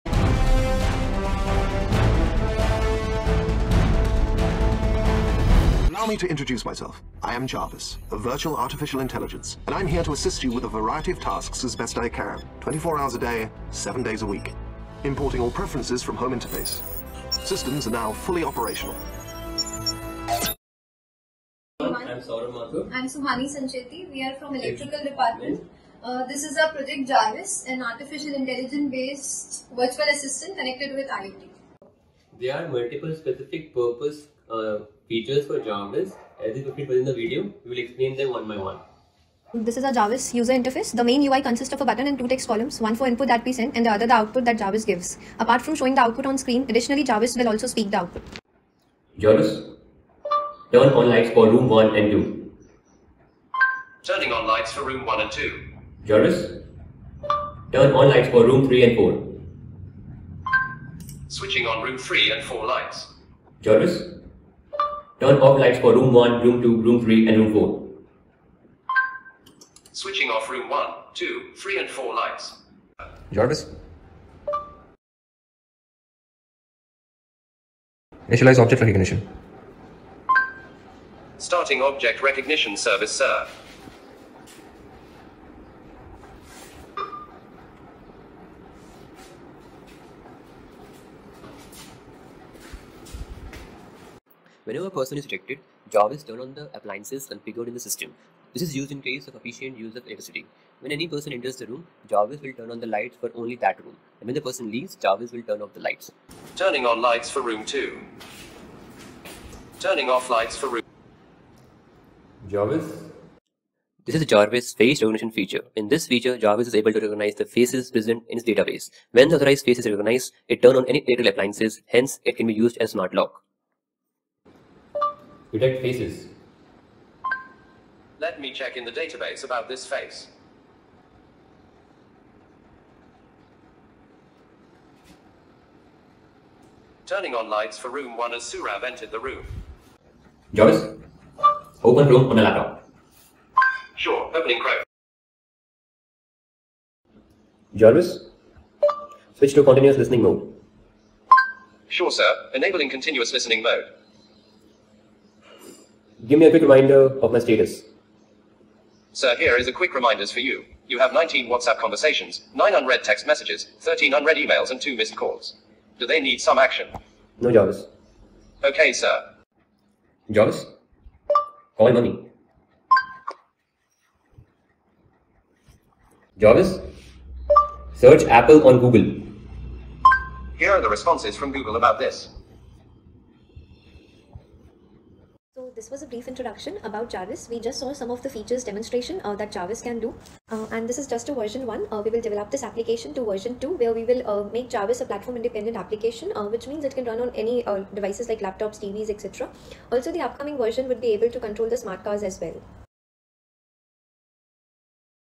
EE07: "J.A.R.V.I.S - An Artificial Intelligence based Virtual | Project | EE | JIET Jodhpur
Assistant connected with the Internet of Things."
JIET Jodhpur Electrical Engineering Final Year Project on "J.A.R.V.I.S - An Artificial Intelligence based Virtual Assistant connected with the Internet of Things." by Sourabh Mathur, Suhani Sancheti